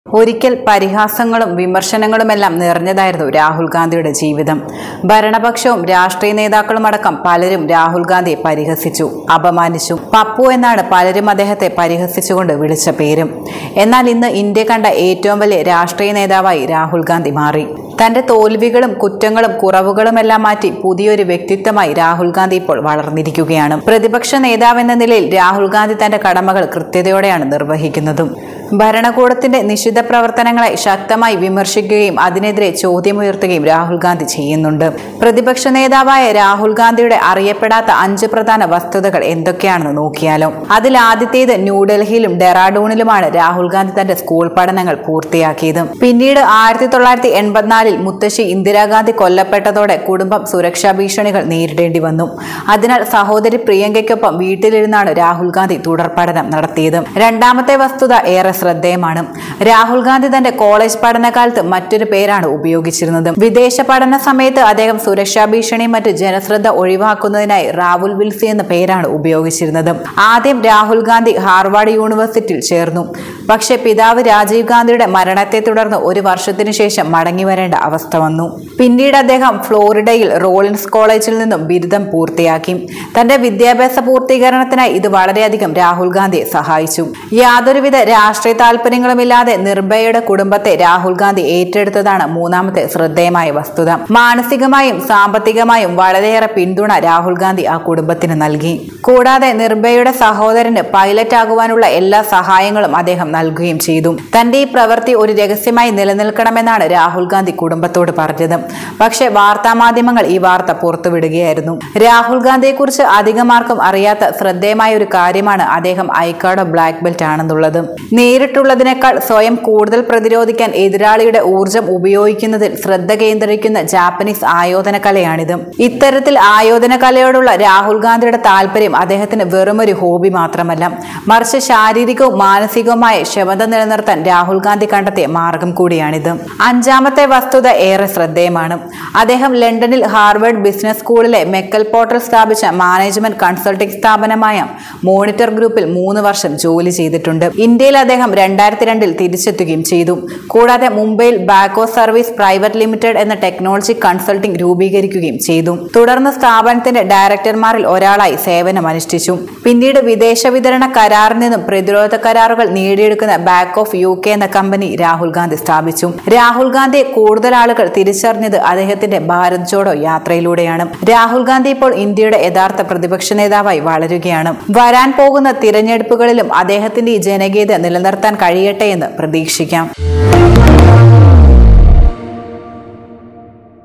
രാഹുൽ ഗാന്ധിയെക്കുറിച്ച് അധികം ആർക്കും അറിയാത്ത 5 വസ്തുതകൾ ഇതാ.. | RAHUL GANDHI
ഒരിക്കൽ പരിഹാസങ്ങളും വിമർശങ്ങളും എല്ലാം നിറഞ്ഞതായിരുന്നു രാഹുൽ ഗാന്ധിയുടെ ജീവിതം.ഭരണപക്ഷവും രാഷ്ട്രീയ നേതാക്കളും അടക്കം പലരും രാഹുൽ ഗാന്ധിയെ പരിഹസിച്ചു,അപമാനിച്ചു,പപ്പു എന്നാണ് പലരും അദ്ദേഹതെ പരിഹസിച്ചുകൊണ്ട് വിളിച്ച പേര്.എന്നാൽ ഇന്ന് ഇന്ത്യ കണ്ട ഏറ്റവും വലിയ രാഷ്ട്രീയ നേതാവായി രാഹുൽ ഗാന്ധി മാറി.തൻ്റെ തോൽവികളും കുറ്റങ്ങളും കുറവുകളും എല്ലാം മാറ്റി പുതിയ ഒരു വ്യക്തിത്വമായി രാഹുൽ ഗാന്ധി ഇപ്പോൾ വളർന്നിരിക്കുകയാണ്.രാഹുൽ ഗാന്ധിയുടെ അധികം ആരും അറിയാത്ത 5 വസ്തുതകൾ അറിഞ്ഞാലോ?